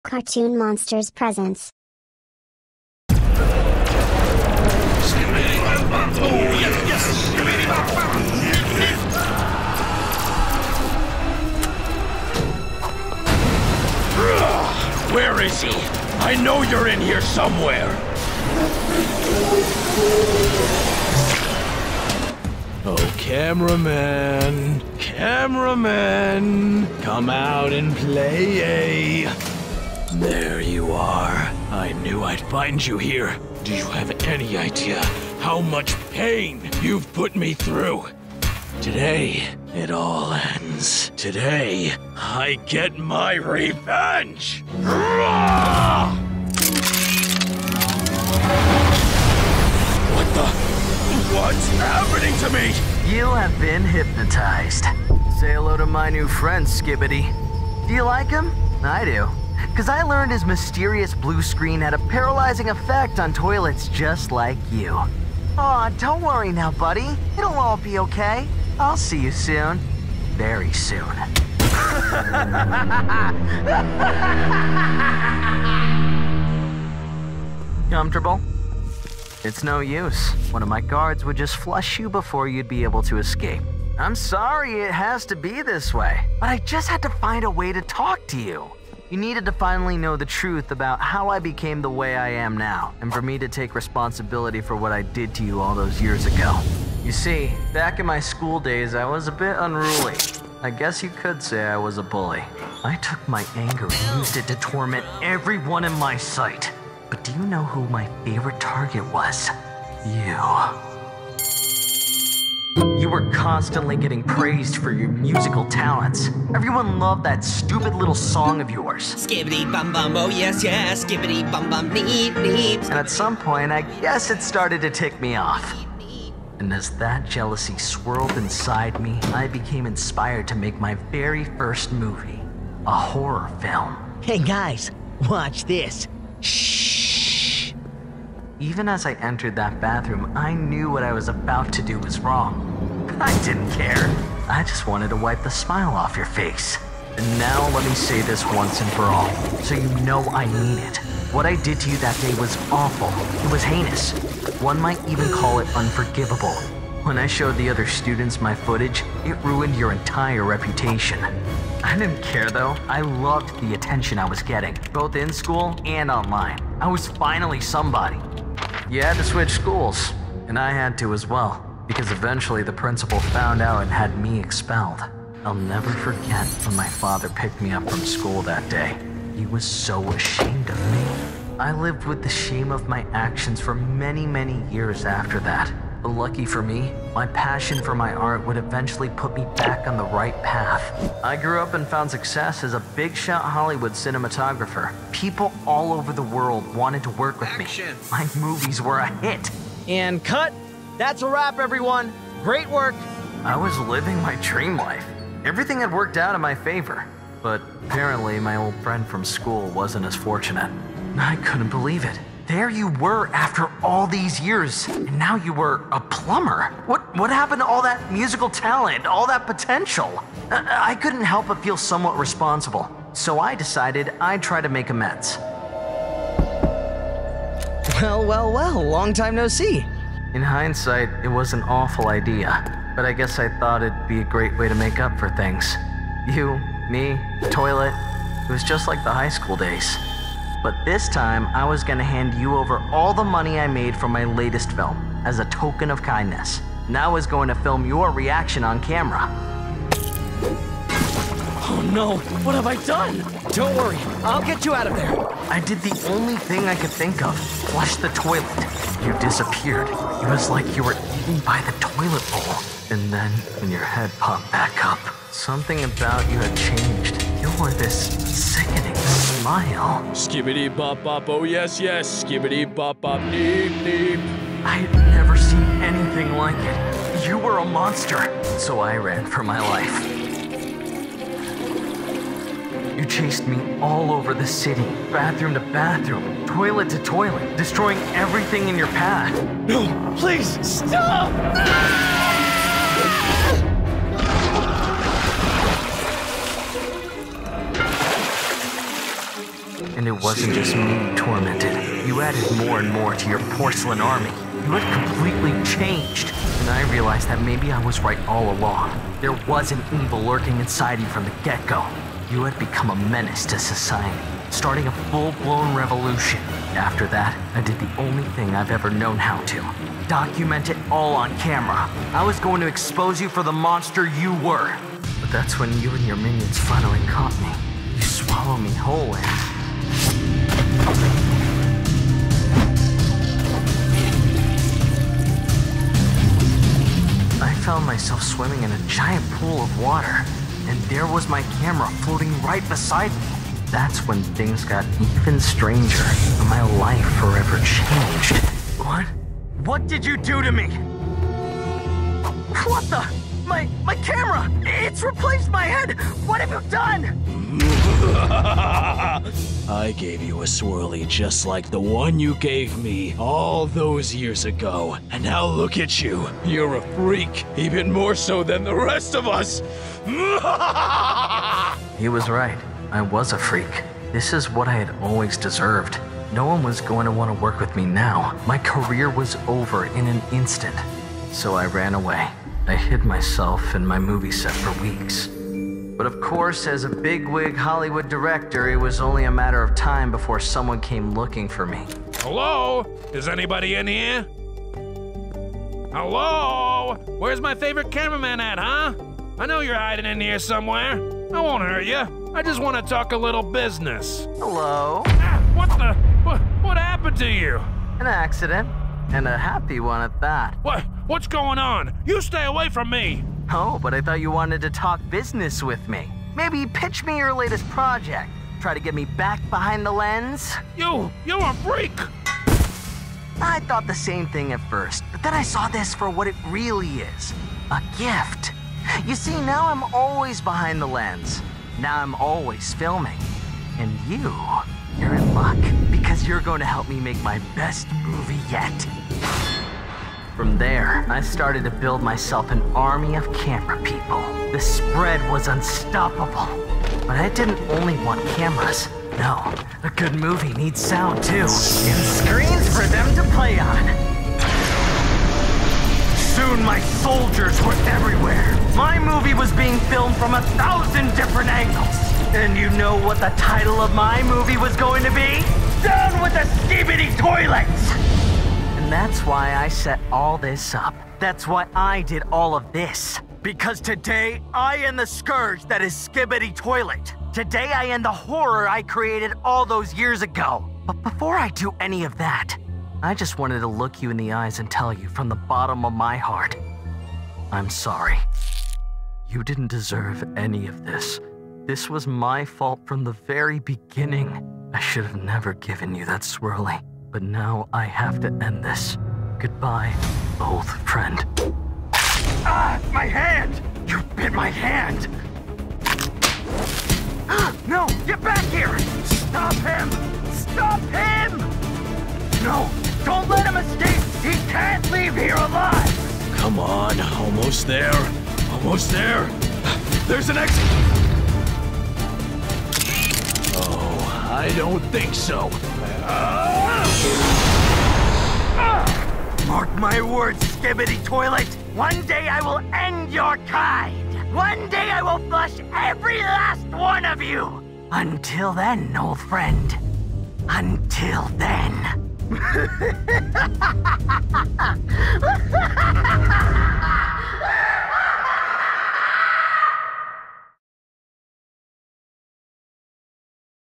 ORIGIN of the CAMERAMAN | CARTOON ANIMATION
In this thrilling YouTube cartoon animation, witness an epic battle of laser-shooting, singing toilets as SKIBIDI TOILET seeks revenge on his former tormentor, CAMERAMAN. Follow the action as SKIBIDI TOILET confronts his nemesis in a high-octane showdown, revealing the dark truth behind their past. With hypnotic powers and an army at his command, the CAMERAMAN will stop at nothing to capture SKIBIDI TOILET and unleash his revenge. Prepare for a mind-bending twist as the CAMERAMAN's quest for redemption takes an unexpected turn, leading to an army of transformed cameramen. Can SKIBIDI TOILET escape his fate and put an end to the CAMERAMAN's sinister plans? Watch this thrilling short film to find out!